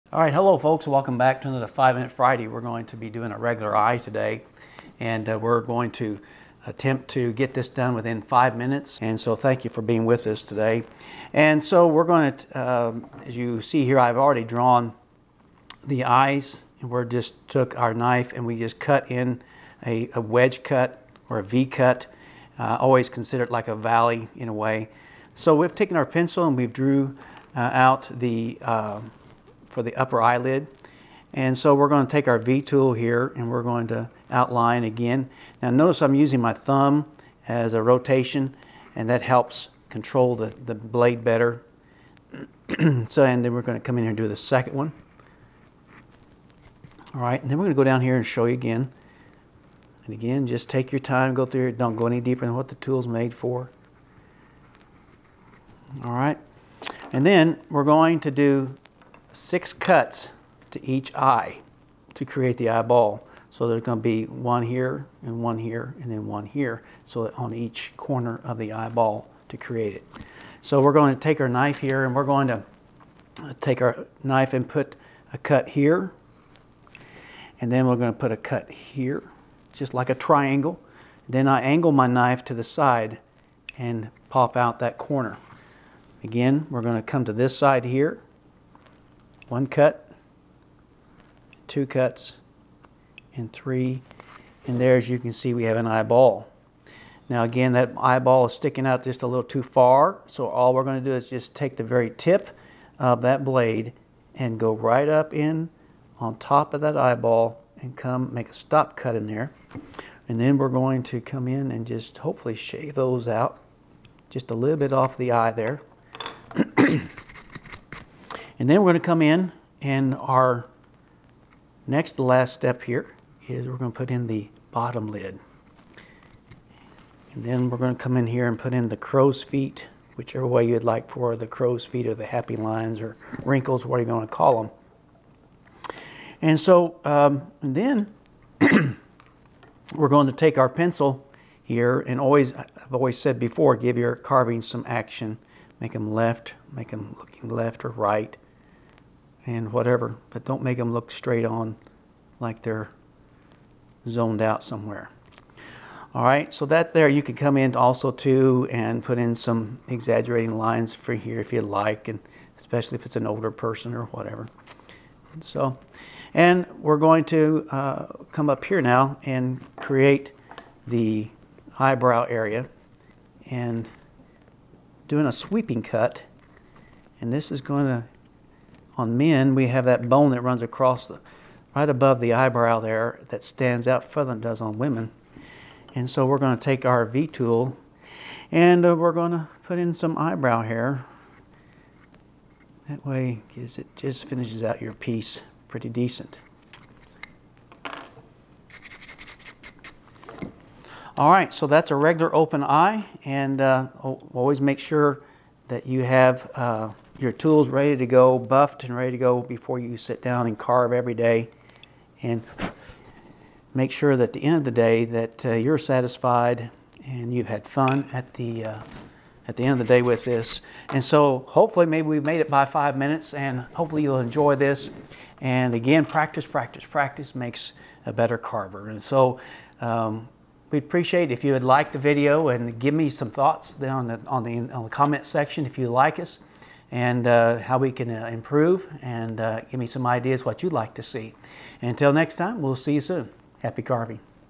Step-By-Step Guide To Carving A Eye (4k UHD)
Eyes can be challenging, BUT also really enhance and bring your woodcarving to life! One of the first thing someone will look at when seeing your carving is the eyes. In this tutorial I go over how to carve an open eye.

HAPPY CARVING!

🎥 Filmed and edited by: @visionfilms3040